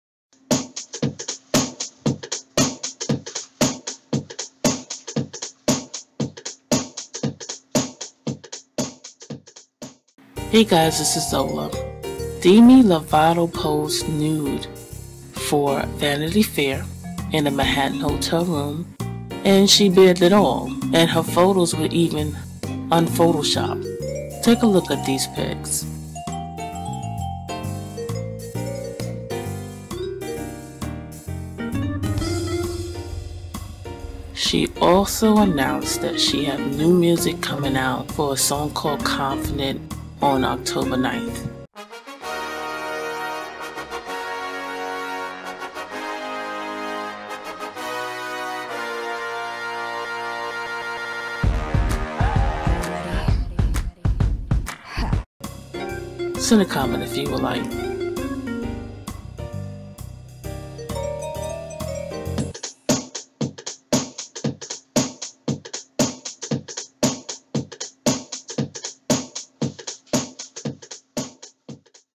DEMI LOVATO Poses Nude for Vanity Fair (PICS)
Demi Lovato has bared her soul - and a lot more - in a new Vanity Fair photoshoot.  The 23-year-old singer was photographed by Patrick Ecclesine at a Manhattan hotel at the end of last month.
In a post on the Vanity Fair website, Ecclesine describes how a vulnerable Lovato was still in mourning after the death of her great-grandfather the day before.

Courtesy of Vanity Fair/Patrick Ecclesine

Yours truly,

CelebrityTV

Demi Lovato, nude, poses nude, Vanity Fair, Demi Lovato poses nude for Vanity Fair, CelebrityTV